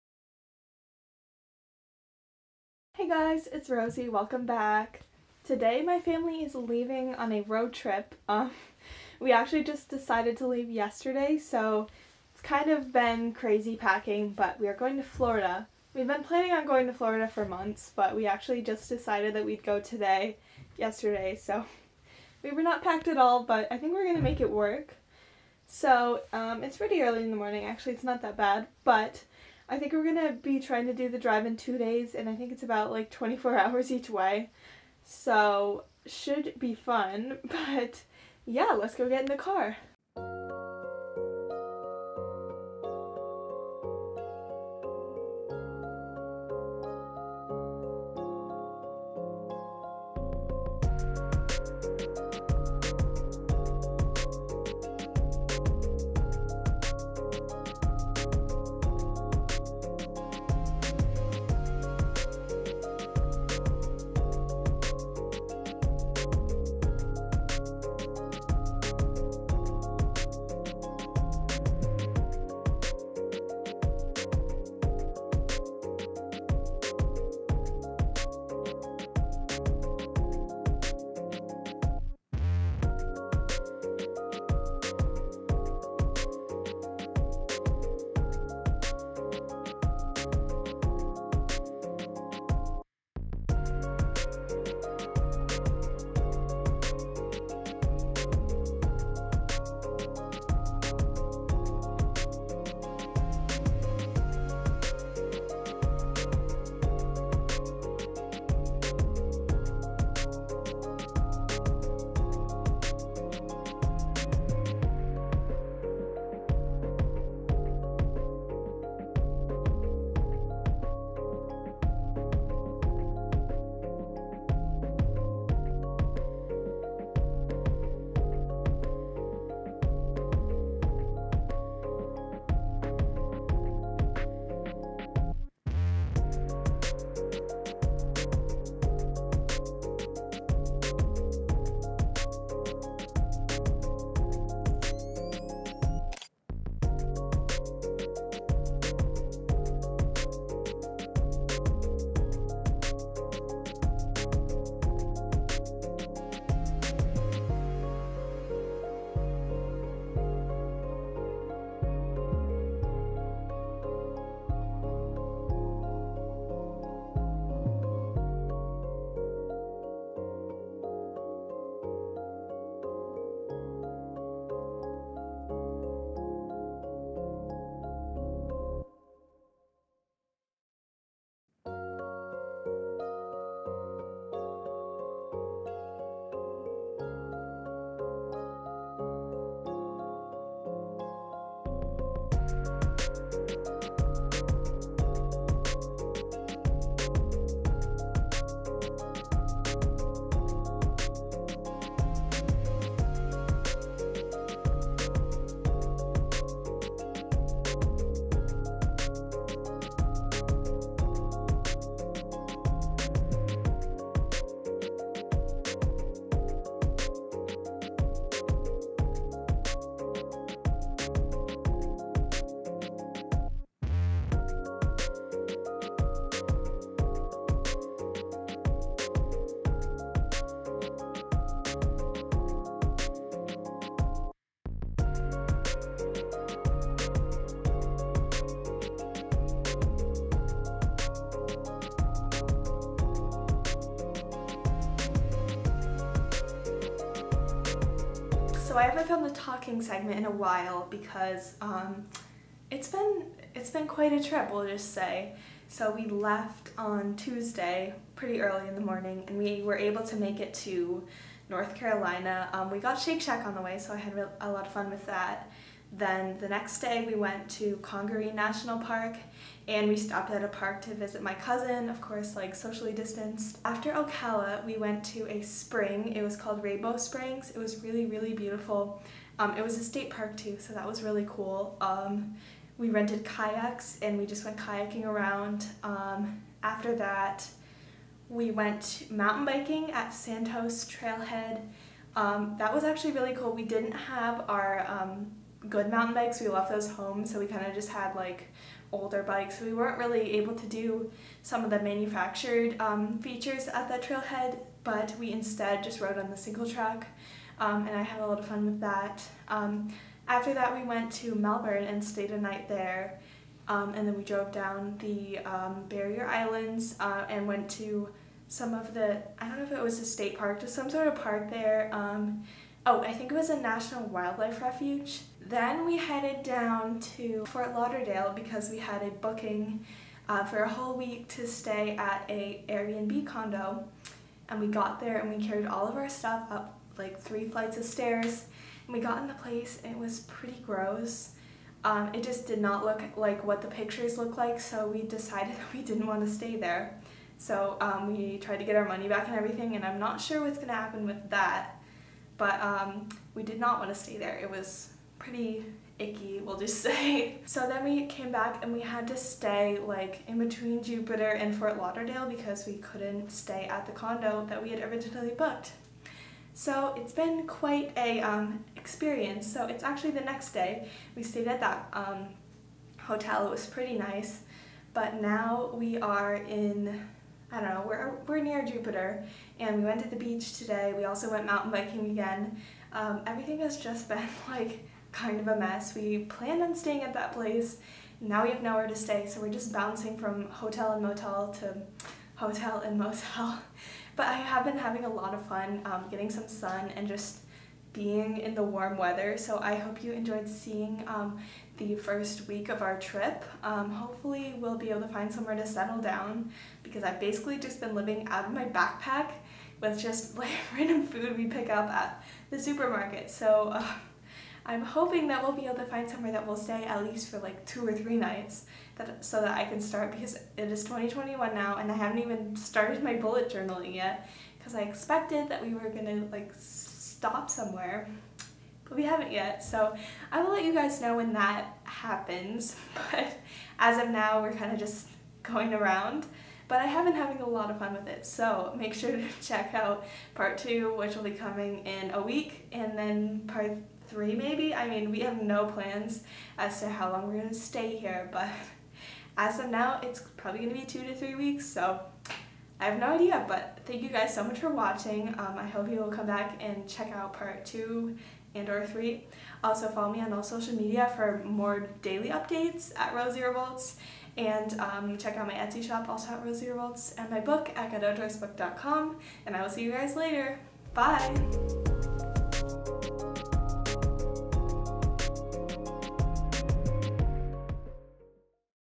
Florida Road Trip! Congaree National Park, Kayaking at Rainbow Springs, Mountain Biking, and more!
Come along on a grand adventure, starting with a 24 hour drive from snowy New Hampshire to warm and sunny Florida. Along the way, see our stop at Congaree National Park, and also experience some of what we have been doing in Florida! Come kayaking at Rainbow Springs State Park, mountain biking at Santos Trailhead State Park, walking at Pelican Island National Wildlife Refuge, and more!